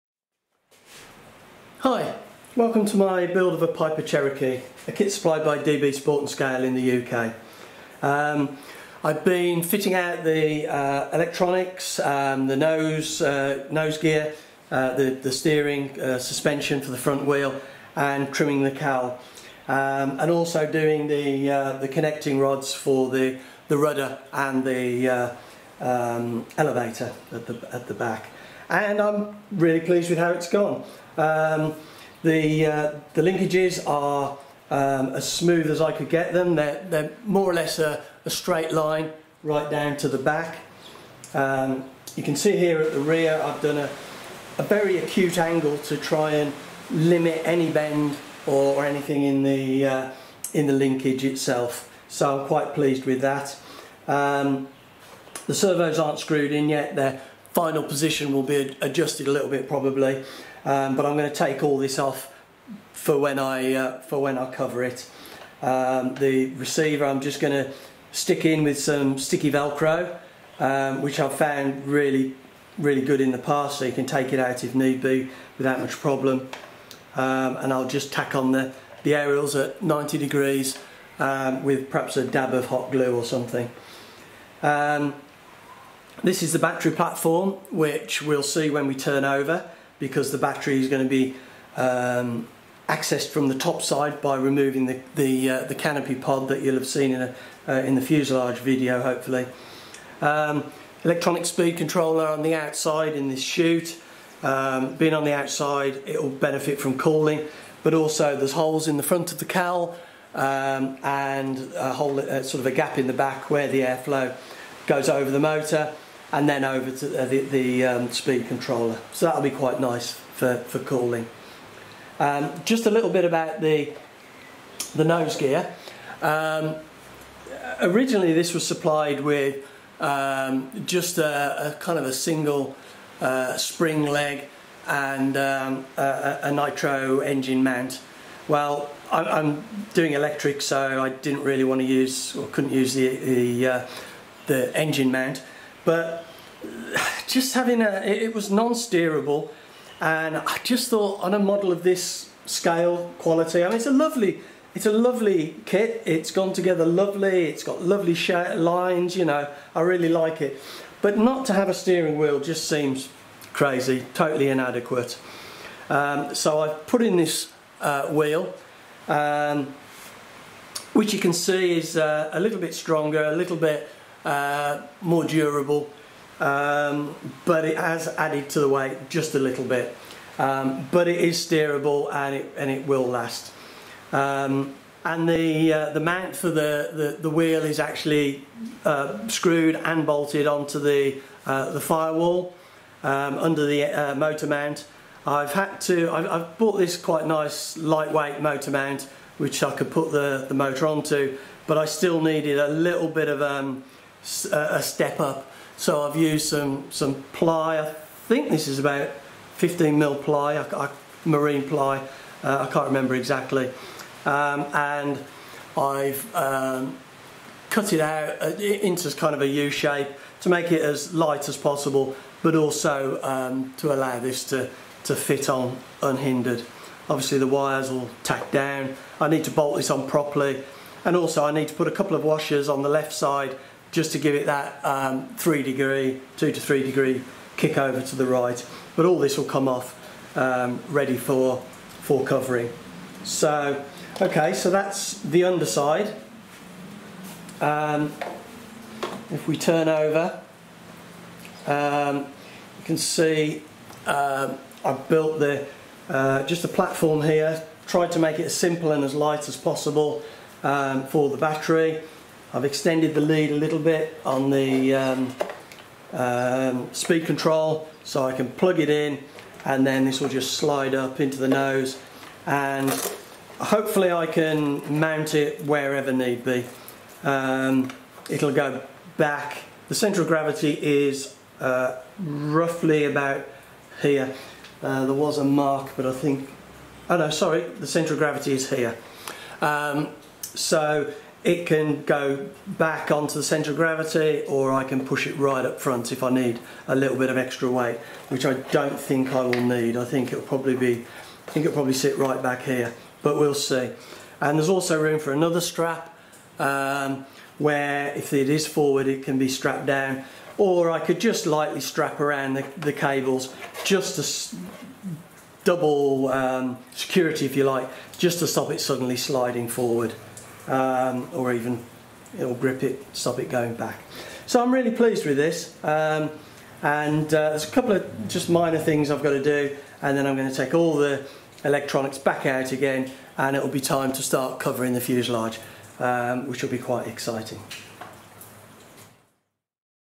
Piper Cherokee RC plane kit - Pt 11: Motor Mount, Electronics & Controls - DB Sport & Scale
Motor Mount, Electronics & Controls the 11th in a series of videos that take you through the unboxing and build of a 56" wingspan Piper Cherokee, a kit from DB Sport & Scale Ltd, in the UK.  This is my first RC plane kit build in about 40 years, so a steep learning curve for me.

HobbyWing Sky Walker 40 Amp-UBEC

3000Mah 4S battery was used

FlySky Sensor Fs-CVT01

FlySky Receiver - FS-iA6B

JX Digital Wing Servos - PDI-2105MG

JX Digital Servos - PDI-2506MG